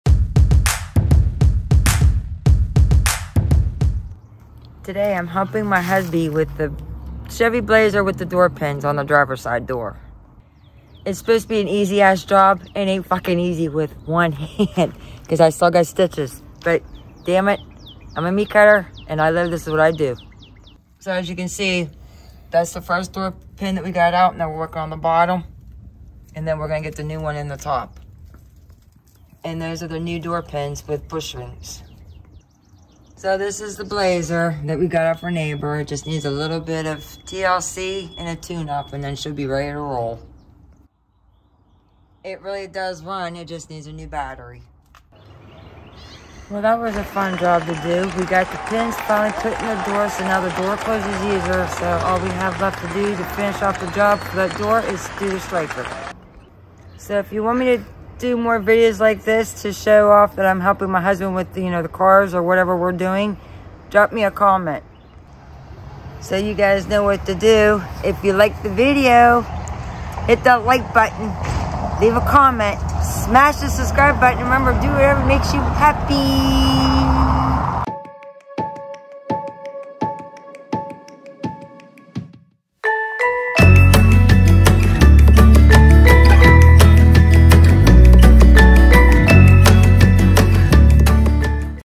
HELPING MY HUBBY WITH THE VEHICLES FIX
WELL I KEPT HOLDING THE DOOR AS HE LAUGHED AND STATED THE JACK IS HOLDING IT I HELPED EVEN WHEN I HAVE MY STITCHES